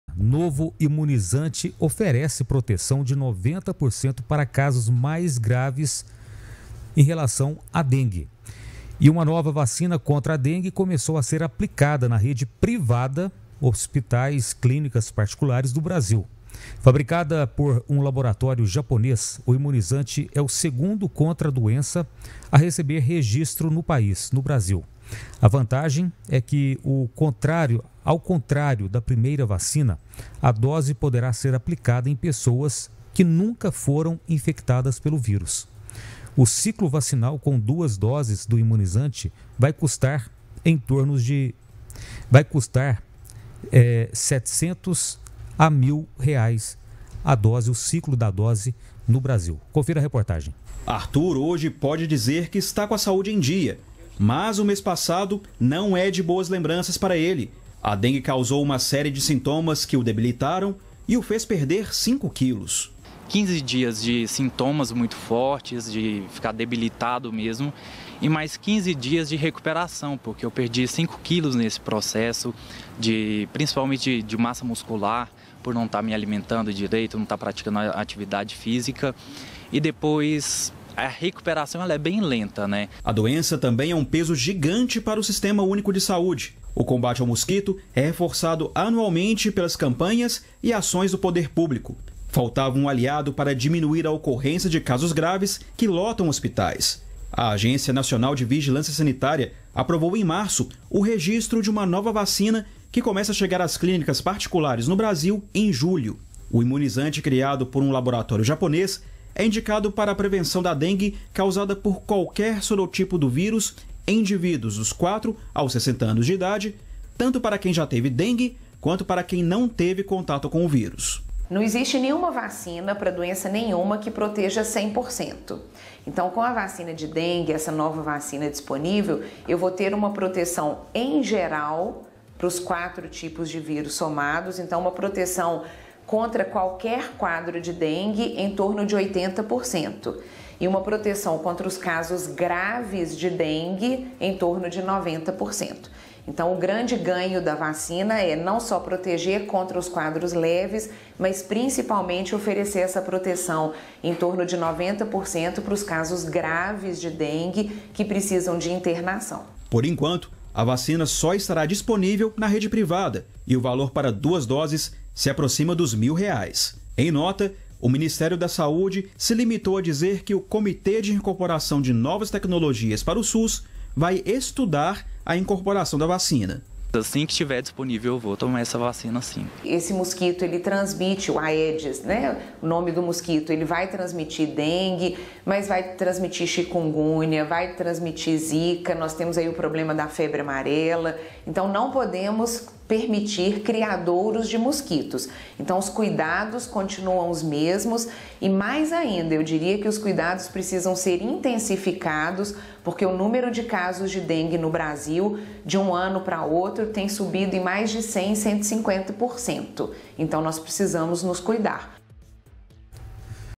NOVA VACINA CONTRA DENGUE OFERECE PROTEÇÃO DE 90% PARA CASOS MAIS GRAVES
| BRASIL |

UMA NOVA VACINA CONTRA A DENGUE COMEÇOU A SER APLICADA NA REDE PRIVADA NO BRASIL. FABRICADA POR UM LABORATÓRIO JAPONÊS, O IMUNIZANTE É O SEGUNDO CONTRA A DOENÇA A RECEBER REGISTRO NO PAÍS. A VANTAGEM É QUE AO CONTRÁRIO DA PRIMEIRA VACINA, A DOSE PODERÁ SER APLICADA EM PESSOAS QUE NUNCA FORAM INFECTADAS PELO VÍRUS. O CICLO VACINAL COM DUAS DOSES DO IMUNIZANTE VAI CUSTAR DE 700 A MIL REAIS. SAIBA MAIS INFORMAÇÕES: